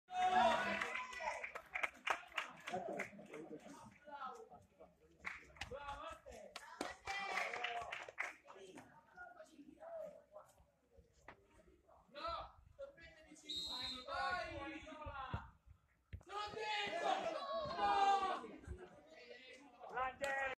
VIRTUS CASTEL FRENTANO " ALLIEVI " Stag. 2017-2018
Domenica 3 Dicembre 2017 , vittoria strepitosa dei nostri ragazzi al " Nuovo " di Via Olimpia contro  il Sambuceto Calcio per la 7^ Giornata di Andata del Campionato Provinciale ALLIEVI , Girone Chieti .
RISULTATO FINALE :  4 - 2 a favore della Virtus Castel Frentano .
MARCATORI :  1^ Tempo  Emanuele Giallonardo detto Lele ,
2^ Tempo rete di Teidi Mata , Lorenzo CUNIBERTI detto Pitbull , e 4 rete ad opera di Bartolomeo CIRILLO detto Bartolo .